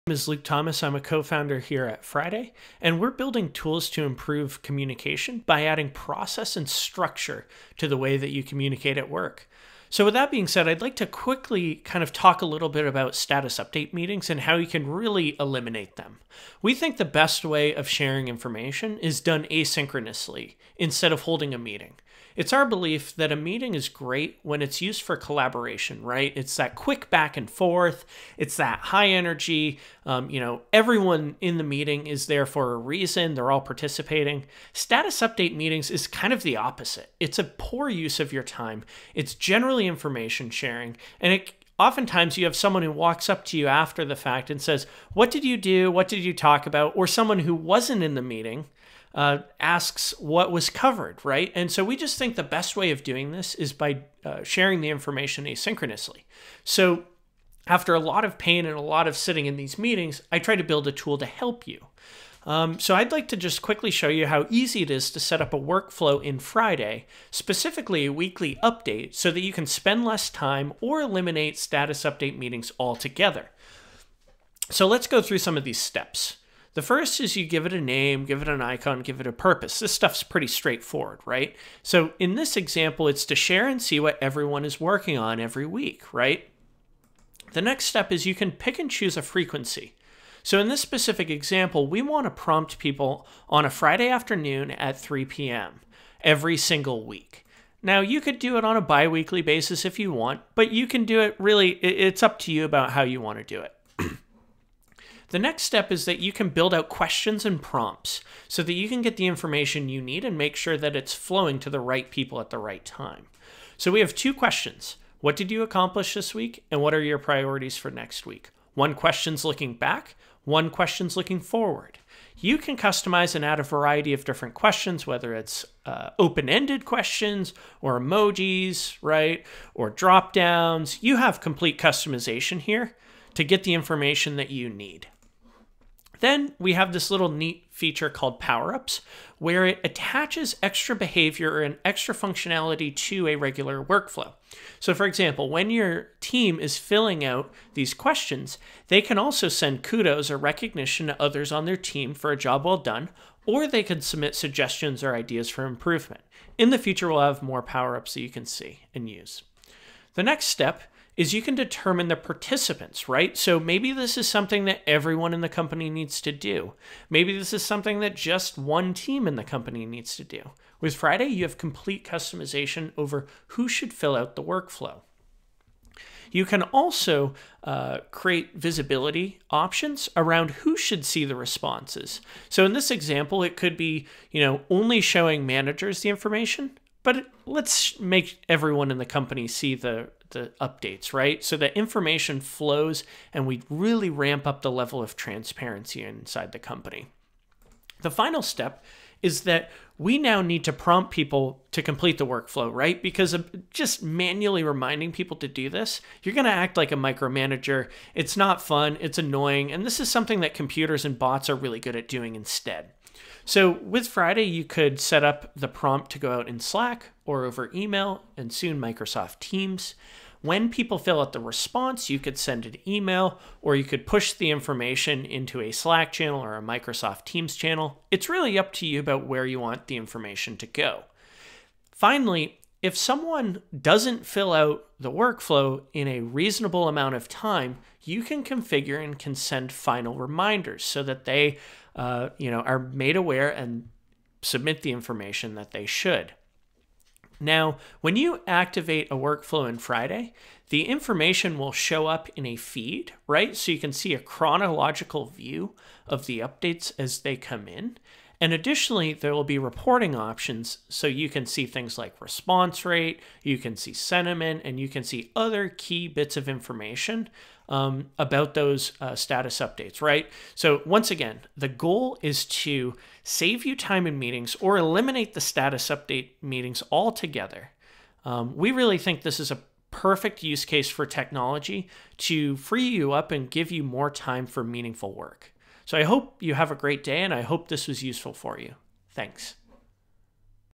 Eliminate boring status update meetings with automatic check-ins
You weren't born to spend all day in boring status update meetings. See how automatic check-ins work.

In this video, we'll show you how Friday.app can help you eliminate status update meetings by sharing the information asynchronously instead.

Transcript below:

 I'm a cofounder here at Friday, and we're building tools to improve communication by adding process and structure to the way that you communicate at work. So with that being said, I'd like to quickly kind of talk a little bit about status update meetings and how you can really eliminate them.

We think the best way of sharing information is done asynchronously. It's that quick back and forth. It's that high energy. Um, you know, everyone in the meeting is there for a reason.

They're all participating. Status update meetings is kind of the opposite. It's a poor use of your time. It's generally information sharing, and it. Oftentimes you have someone who walks up to you after the fact and says, what did you do? What did you talk about? Or someone who wasn't in the meeting, uh, asks what was covered.

Right? And so we just think the best way of doing this is by sharing the information asynchronously. So. After a lot of pain and a lot of sitting in these meetings, I tried to build a tool to help you. Um, so I'd like to just quickly show you how easy it is to set up a workflow in Friday, specifically weekly updates so that you can spend less time or eliminate status update meetings altogether.

So let's go through some of these steps. The first is you give it a name, give it an icon, give it a purpose. This stuff's pretty straightforward, right? So in this example, it's to Sharon, see what everyone is working on every week, right? The next step is you can pick and choose a frequency. So in this specific example, we want to prompt people on a Friday afternoon at 3:00 PM every single week.

Now, you could do it on a biweekly basis if you want, but you can do it. Really, it's up to you about how you want to do it. The next step is that you can build out questions and prompts so that you can get the information you need and make sure that it's flowing to the right people at the right time.

So we have two questions. What did you accomplish this week and what are your priorities for next week? One question is looking back one questions. Looking forward, you can customize and add a variety of different questions, whether it's. Uh, open ended questions or emojis, right? Or drop downs.

You have complete customization here to get the information that you need. Then we have this little neat feature called power ups where it attaches extra behavior or an extra functionality to a regular workflow. So for example, when your team is filling out these questions, they can also send kudos or recognition others on their team for a job well done.

Or they could submit suggestions or ideas for improvement in the future, we'll have more power up so you can see and use. The next step is you can determine the participants, right? So maybe this is something that everyone in the company needs to do. Maybe this is something that just one team in the company needs to do was Friday you have complete customization over who should fill out the workflow.

You can also, uh, create visibility options around who should see the responses. So in this example, it could be, you know, only showing managers the information, but let's make everyone in the company see the updates, right? So the information flows and we really ramp up the level of transparency inside the company.

The final step is that we now need to prompt people to complete the workflow, right? Because just manually reminding people to do this, you're going to act like a micromanager. It's not fun. It's annoying. And this is something that computers and bots are really good at doing instead. So with Friday, you could set up the prompt to go out in Slack.

Or over email and soon Microsoft teams, when people fill out the response, you could send an email or you could push the information into a Slack channel or a Microsoft teams channel. It's really up to you about where you want the information to go. Finally, if someone doesn't fill out the workflow in a reasonable amount of time, you can configure and consent final reminders so that they, uh, you know, are made aware and submit the information that they should.

Now when you activate a workflow in Friday, the information will show up in a feed, right? So you can see a chronological view of the updates as they come in. And additionally, there'll be reporting options.